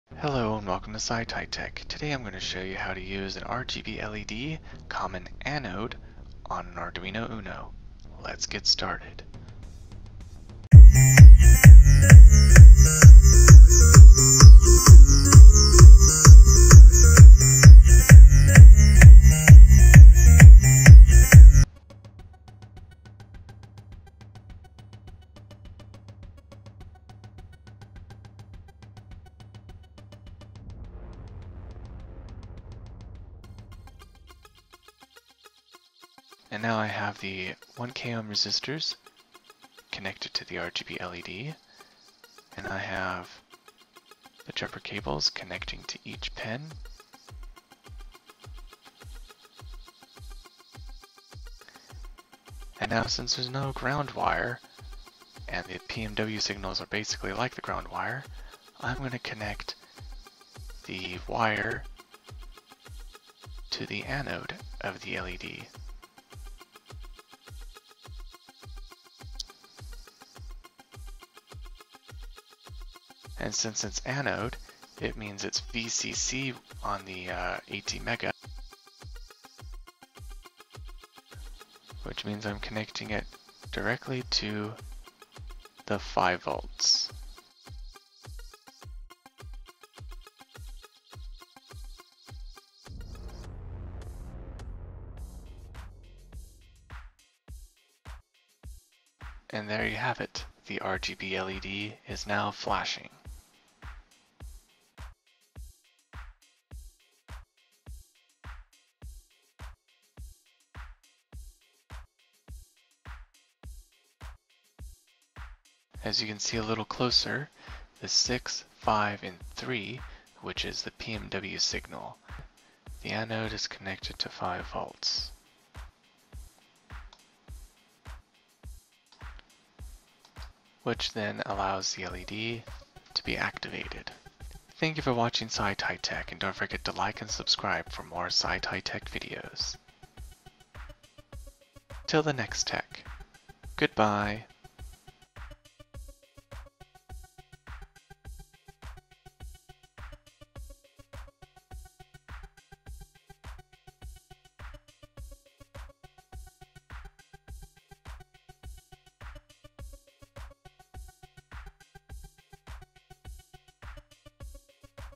RGB LED Common Anode Arduino
This is how you can control a RGB LED common anode with an Arduino.

int 6 =6;
int 5 =5;
int 3 =3;

void setup (){
  pinMode (6,OUTPUT);
  pinMode (5,OUTPUT);
  pinMode (3,OUTPUT);

void loop () {
  digitalWrite(6, HIGH);
  delay(500);
  digitalWrite(5, HIGH);
  delay(500);
  digitalWrite(3, HIGH);
  delay(500);
  digitalWrite(6, LOW);
  delay(1000);
  digitalWrite(5, LOW);
  delay(1000);
  digitalWrite(3, LOW);
  delay(1000);